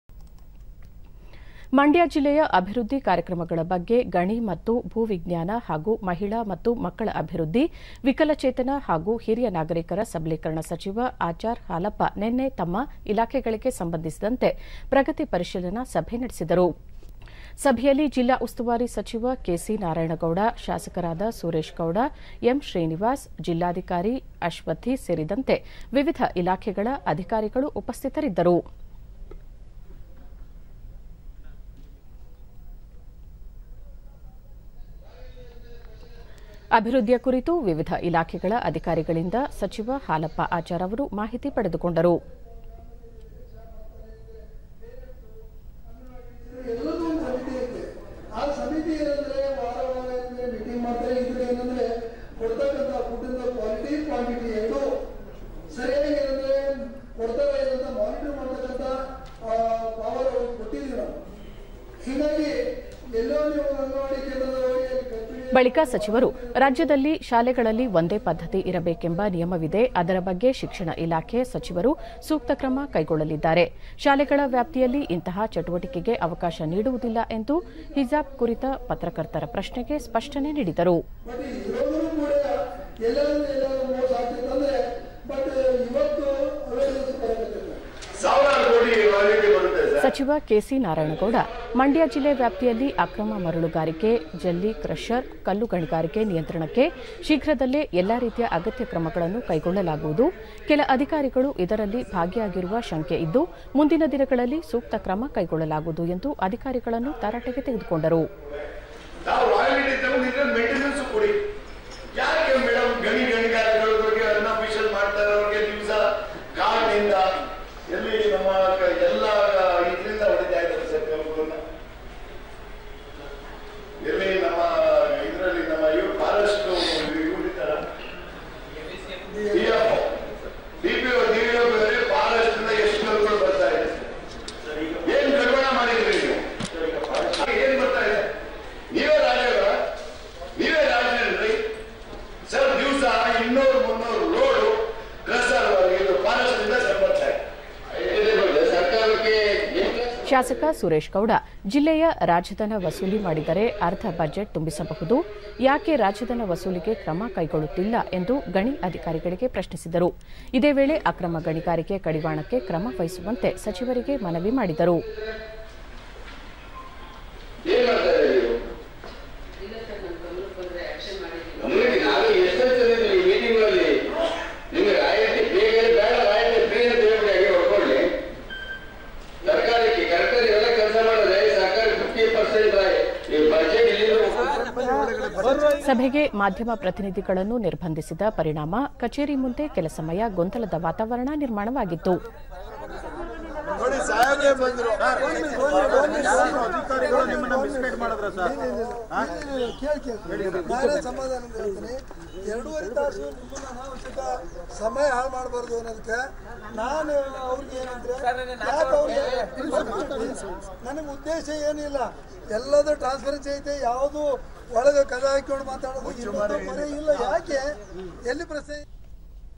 ಮಂಡ್ಯದಲ್ಲಿ ಪ್ರಗತಿ ಪರಿಶೀಲನಾ ಸಭೆ ನಡೆಸಿದ ಸಚಿವ ಆಚಾರ್ ಹಾಲಪ್ಪ ಅಕ್ರಮ ಗಣಿಗಾರಿಕೆ ತಡೆಗೆ ಸಚಿವರಿಗೆ ಶಾಸಕರ ಮನವಿ
ಮಂಡ್ಯ ಜಿಲ್ಲೆಯ  ಅಭಿವೃದ್ಧಿ  ಕಾರ್ಯಕ್ರಮಗಳ ಬಗ್ಗೆ  ಗಣಿ ಮತ್ತು ಭೂ ವಿಜ್ಞಾನ ಹಾಗೂ ಮಹಿಳಾ ಮತ್ತು ಮಕ್ಕಳ ಅಭಿವೃದ್ಧಿ, ವಿಕಲಚೇತನ ಹಾಗೂ ಹಿರಿಯನಾಗರಿಕರ ಸಬಲೀಕರಣ ಸಚಿವ ಆಚಾರ್‌ ಹಾಲಪ್ಪ ನಿನ್ನೆ ತಮ್ಮ ಇಲಾಖೆಗಳಿಗೆ ಸಂಬಂಧಿಸಿದಂತೆ ಪ್ರಗತಿ ಪರಿಶೀಲನಾ ಸಭೆ ನಡೆಸಿದರು.
ಸಭೆಯಲ್ಲಿ ಜಿಲ್ಲಾ ಉಸ್ತುವಾರಿ ಸಚಿವ ಕೆ.ಸಿ.ನಾರಾಯಣಗೌಡ, ಶಾಸಕರಾದ ಸುರೇಶ್ ಗೌಡ,  ಎಂ.ಶ್ರೀನಿವಾಸ್‌, ಜಿಲ್ಲಾಧಿಕಾರಿ ಅಶ್ವಥಿ ಸೇರಿದಂತೆ ವಿವಿಧ ಇಲಾಖೆಗಳ ಅಧಿಕಾರಿಗಳು ಉಪಸ್ಥಿತರಿದ್ದರು.
ಅಭಿವೃದ್ಧಿಯ ಕುರಿತು ವಿವಿಧ ಇಲಾಖೆಗಳ ಅಧಿಕಾರಿಗಳಿಂದ ಸಚಿವ ಹಾಲಪ್ಪ ಆಚಾರ್‌ ಅವರು ಮಾಹಿತಿ ಪಡೆದುಕೊಂಡರು.

ಬಳಿಕ ಸಚಿವರು, ರಾಜ್ಯದಲ್ಲಿ ಶಾಲೆಗಳಲ್ಲಿ ಒಂದೇ ಪದ್ಧತಿ ಇರಬೇಕೆಂಬ ನಿಯಮವಿದೆ ಅದರ ಬಗ್ಗೆ ಶಿಕ್ಷಣ ಇಲಾಖೆ ಸಚಿವರು ಸೂಕ್ತ ಕ್ರಮಕೈಗೊಳ್ಳಲಿದ್ದಾರೆ. ಶಾಲೆಗಳ ವ್ಯಾಪ್ತಿಯಲ್ಲಿ  ಇಂತಹ ಚಟುವಟಿಕೆಗಳಿಗೆ ಅವಕಾಶ ನೀಡುವುದಿಲ್ಲ ಎಂದು ಹಿಜಾಬ್ ಕುರಿತ ಪತ್ರಕರ್ತರ ಪ್ರಶ್ನೆಗೆ ಸ್ಪಷ್ಟನೆ ನೀಡಿದರು.

ಸಚಿವ ಕೆ.ಸಿ.ನಾರಾಯಣಗೌಡ, ಮಂಡ್ಯ ಜಿಲ್ಲೆ ವ್ಯಾಪ್ತಿಯಲ್ಲಿ ಅಕ್ರಮ ಮರಳುಗಾರಿಕೆ, ಜಲ್ಲಿ ಕ್ರಷರ್‌, ಕಲ್ಲು ಗಣಿಗಾರಿಕೆ ನಿಯಂತ್ರಣಕ್ಕೆ ಶೀಘ್ರದಲ್ಲೇ ಎಲ್ಲಾ ರೀತಿಯ ಅಗತ್ಯ ಕ್ರಮಗಳನ್ನು ಕೈಗೊಳ್ಳಲಾಗುವುದು. ಕೆಲ ಅಧಿಕಾರಿಗಳು ಇದರಲ್ಲಿ ಭಾಗಿಯಾಗಿರುವ ಶಂಕೆಯಿದ್ದು ಮುಂದಿನ ದಿನಗಳಲ್ಲಿ ಸೂಕ್ತ ಕ್ರಮಗೊಗೊಳ್ಳಲಾಗುವುದು ಎಂದು ಅಧಿಕಾರಿಗಳನ್ನು ತರಾಟೆಗೆ ತೆಗೆದುಕೊಂಡರು.
ಶಾಸಕ ಸುರೇಶ್‌ ಗೌಡ, ಜಿಲ್ಲೆಯ ರಾಜಧನ ವಸೂಲಿ ಮಾಡಿದರೆ ಅರ್ಧ ಬಜೆಟ್‌ ತುಂಬಿಸಬಹುದು, ಯಾಕೆ ರಾಜಧನ ವಸೂಲಿಗೆ ಕ್ರಮಕೈಗೊಳ್ಳುತ್ತಿಲ್ಲ ಎಂದು ಗಣಿ ಅಧಿಕಾರಿಗೆ ಪ್ರಶ್ನಿಸಿದರು. ಇದೇ ವೇಳೆ ಅಕ್ರಮ ಗಣಿಗಾರಿಕೆ ಕಡಿವಾಣಕ್ಕೆ ಕ್ರಮವಹಿಸುವಂತೆ ಸಚಿವರಿಗೆ ಮನವಿ ಮಾಡಿದರು.
ಸಭೆಗೆ ಮಾಧ್ಯಮ ಪ್ರತಿನಿಧಿಗಳನ್ನು ನಿರ್ಭಂದಿಸಿದ ಪರಿಣಾಮ ಕಚೇರಿ ಮುಂದೆ ಕೆಲ ಸಮಯ ಗೊಂದಲದ ವಾತಾವರಣ ನಿರ್ಮಾಣವಾಗಿತ್ತು.